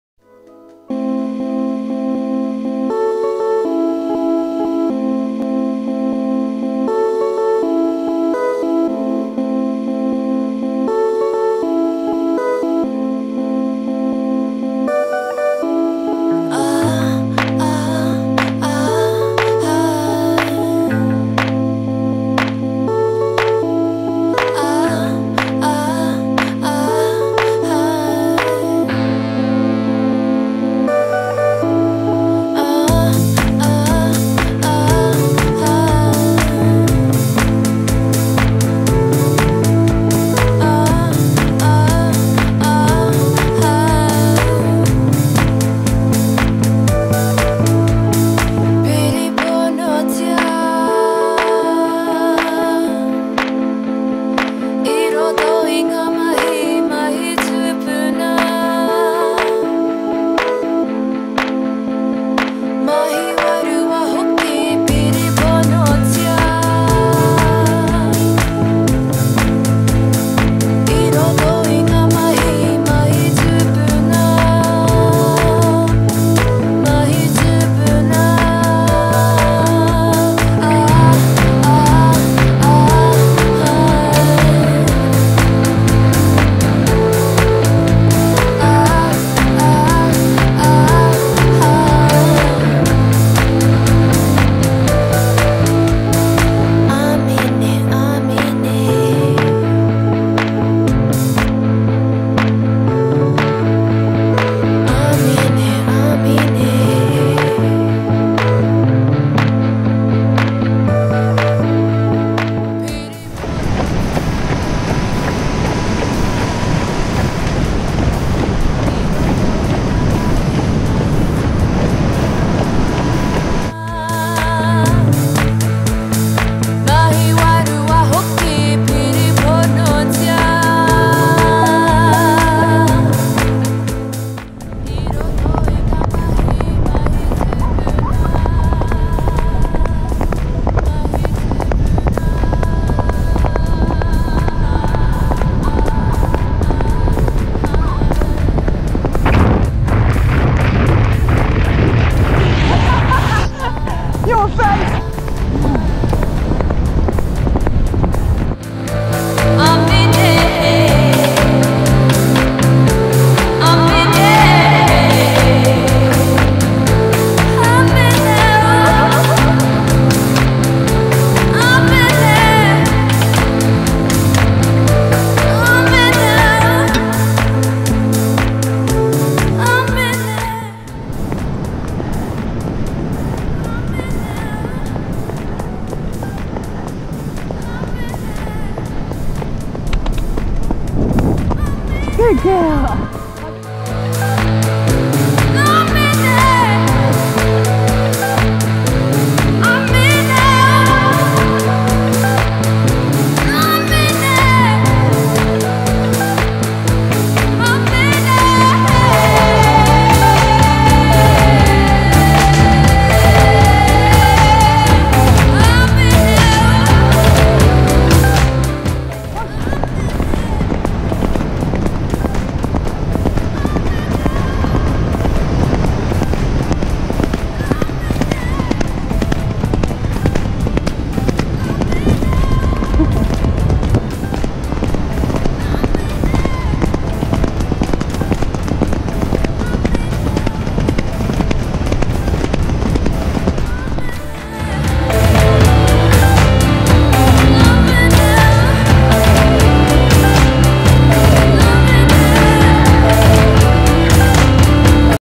Horse Riding on the Beach in Papamoa, Bay of Plenty, New Zealand. Using Go Pro
Go Pro footage of horses being taken to the beach for a ride in Papamoa, Bay of Plenty, New Zealand. Shows jumping logs, riding in the sea and going for a gallop.